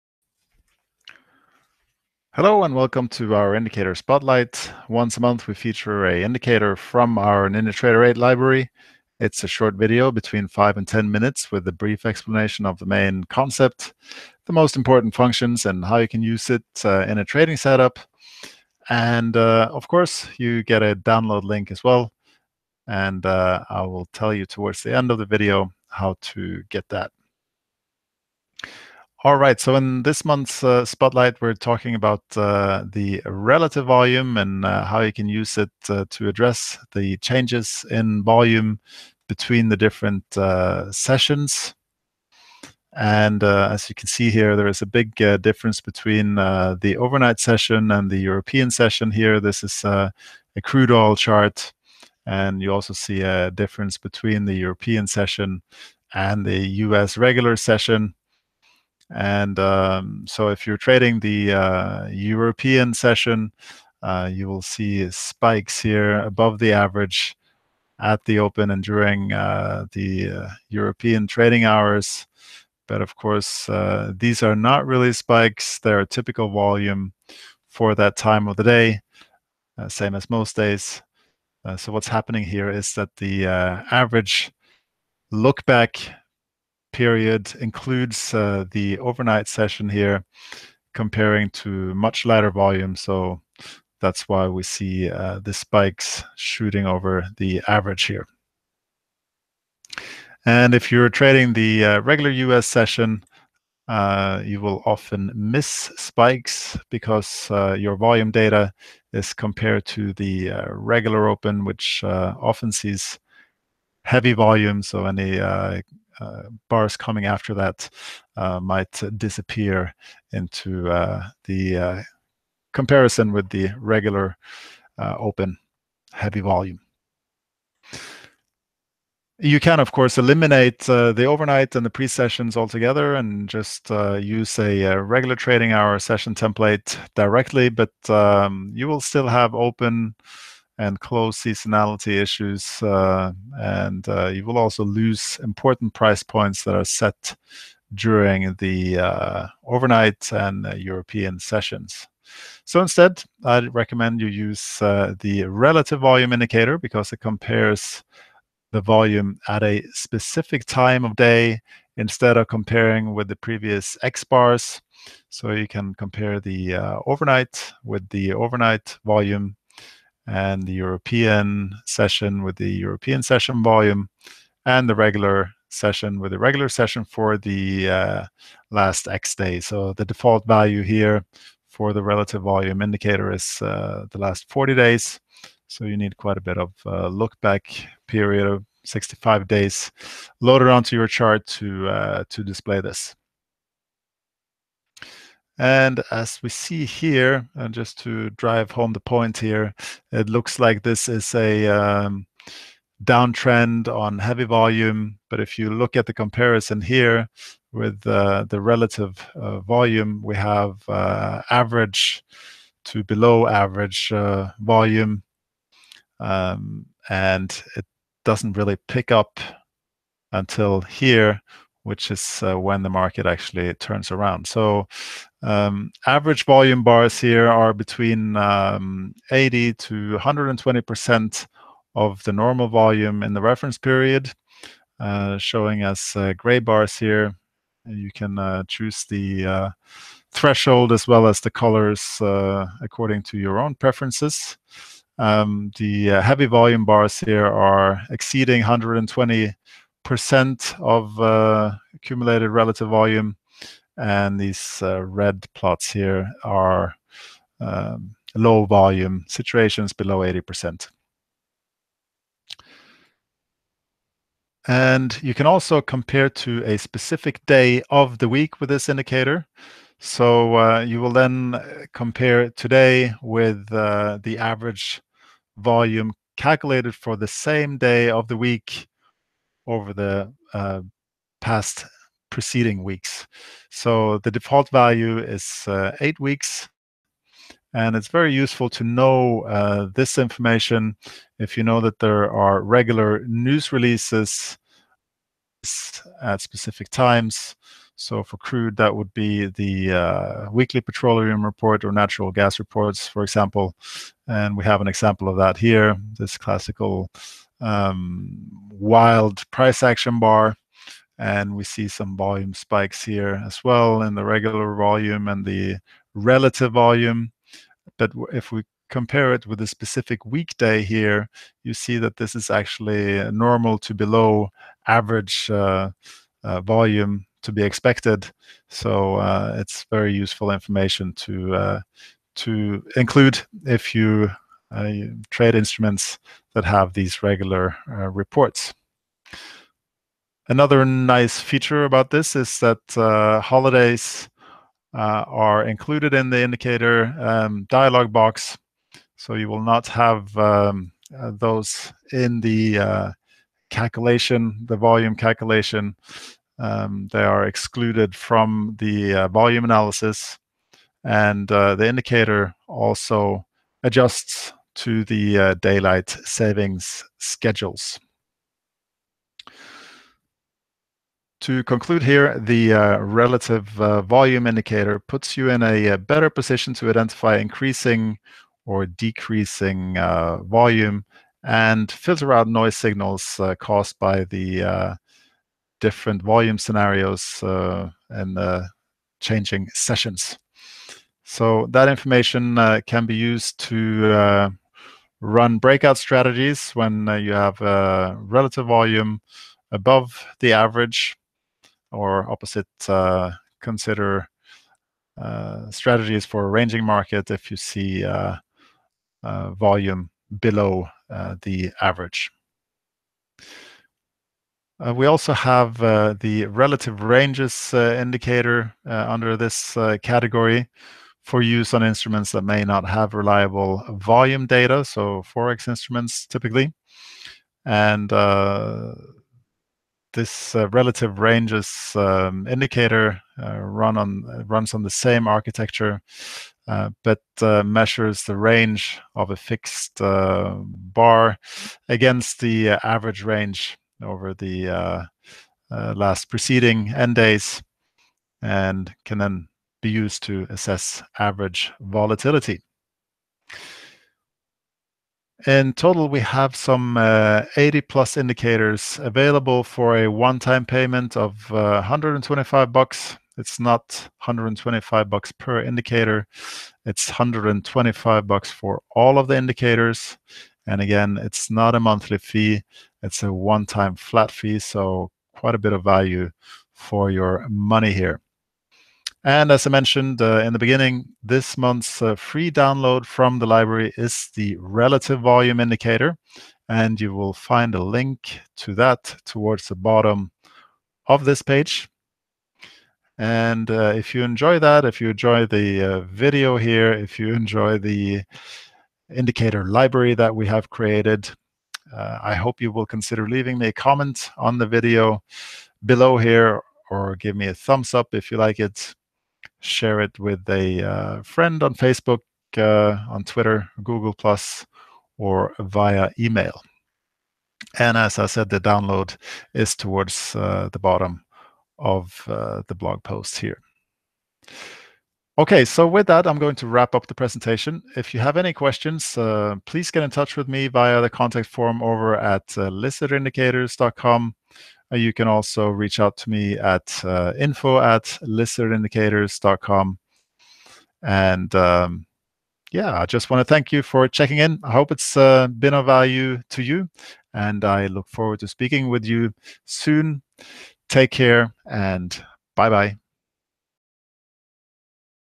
Relative Volume Indicator Spotlight
00:00 Spotlight Intro
00:30 Concept
02:30 Volume on specific times of day
04:25 Volume on specific week days
06:10 Conclusion
06:54 Other volume indicators from the Library

The Relative Volume is a powerful indicator, allowing you to address volume changes between the overnight, European and US regular sessions.